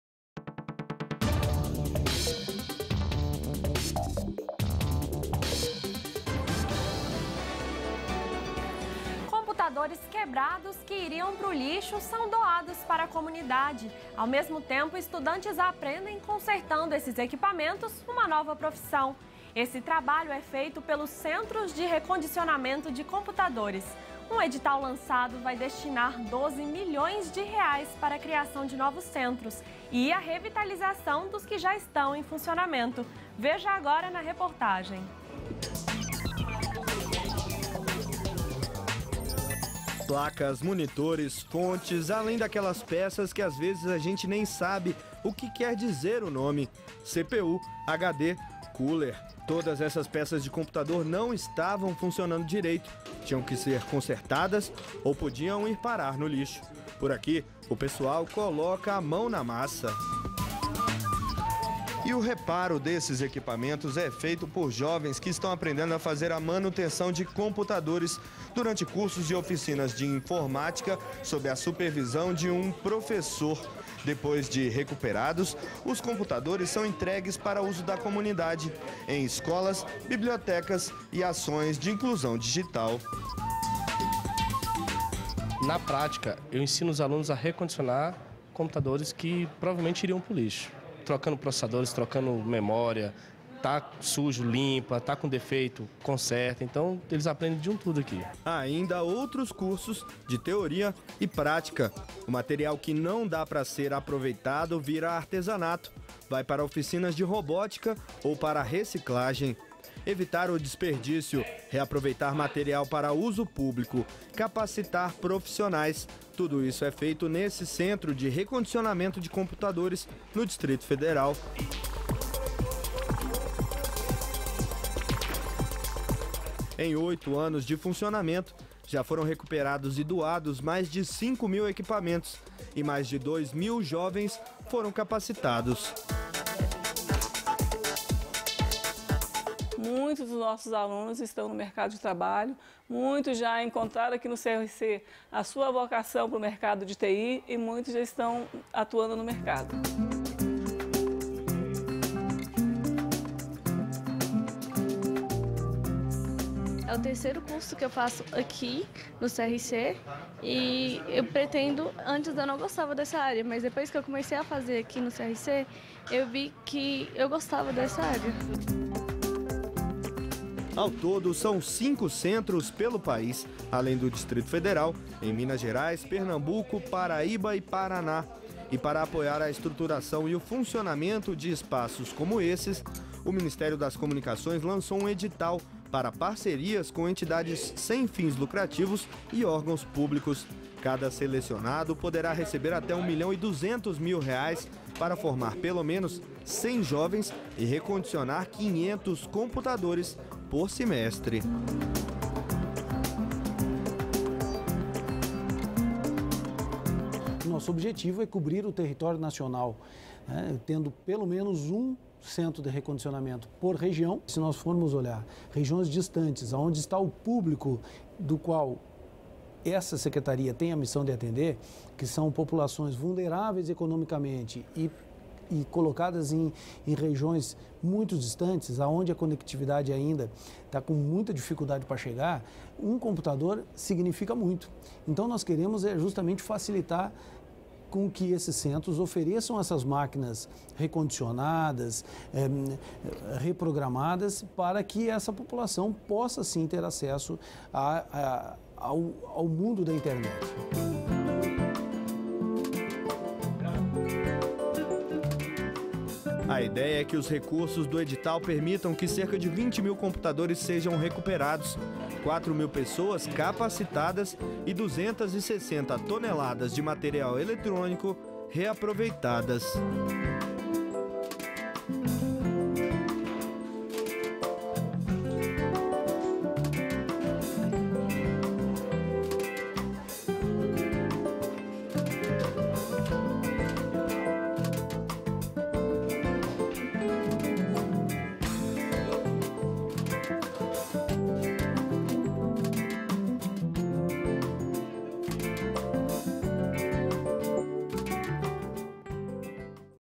Centros de Recondicionamento de Computadores promove capacitação de jovens de baixa renda
DA REDAÇÃO - 05.10.15: Os Centros de Recondicionamento de Computadores (CRCs) fazem parte do projeto Computadores para Inclusão, que promove a formação de jovens de baixa renda. A capacitação é feita por meio de oficinas, cursos e outras atividades oferecidas pelos CRCs. Os computadores quebrados que iriam para o lixo são consertados e doados para a comunidade. Ao mesmo tempo, os estudantes aprendem, consertando esses equipamentos, uma nova profissão.